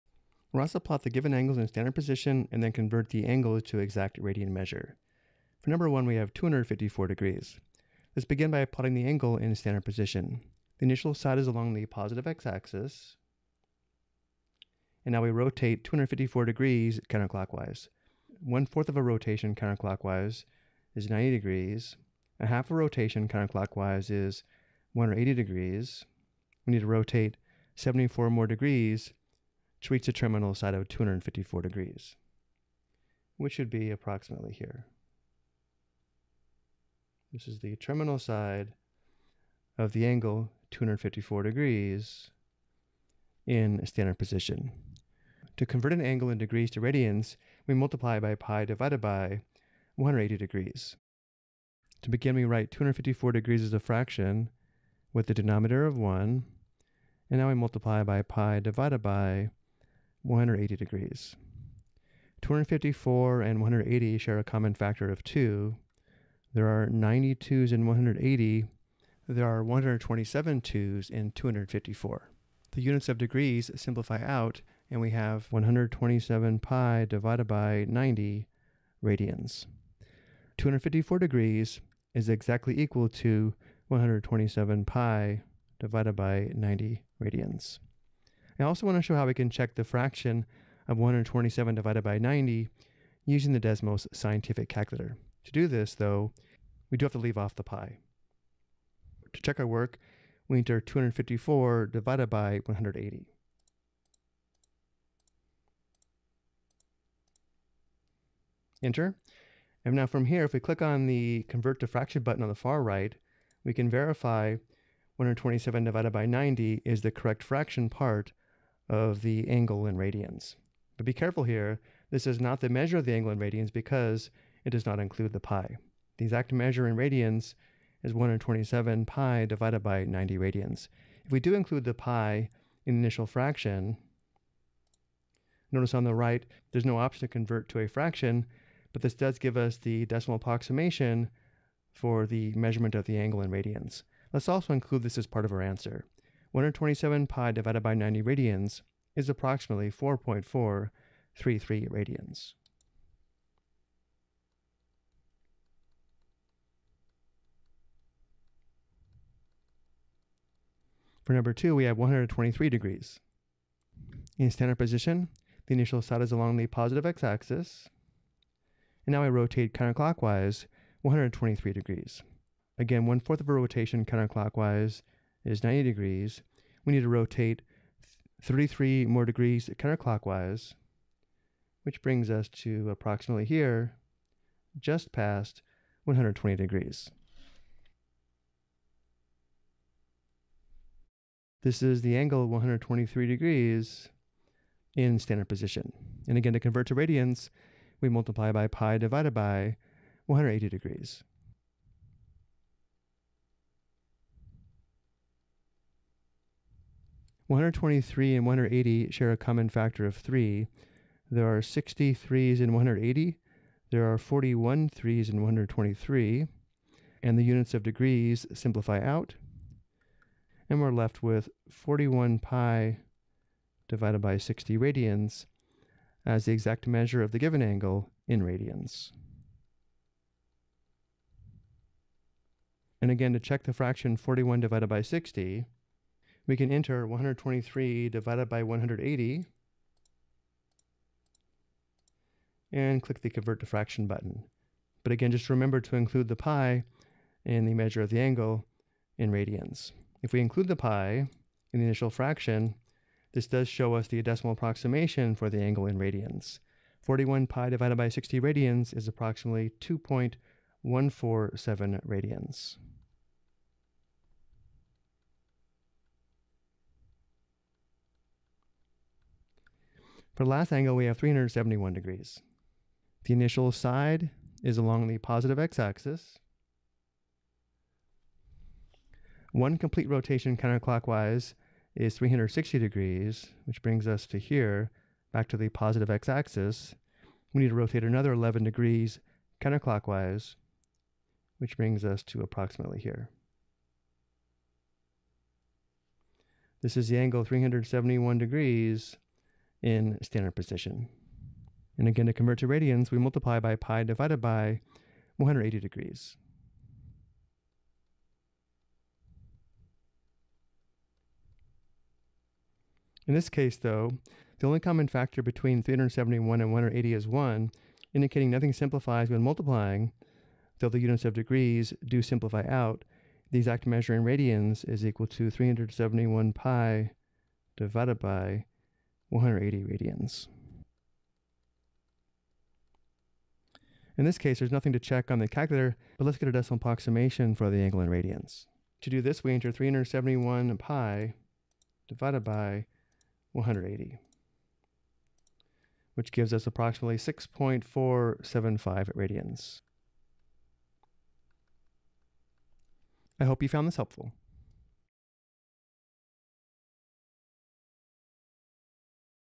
Plot Positive Angles in Degrees in Standard Position and Convert to Radians
This video explains how to plot angles in degrees in standard position and then convert the degree measure to radian measure.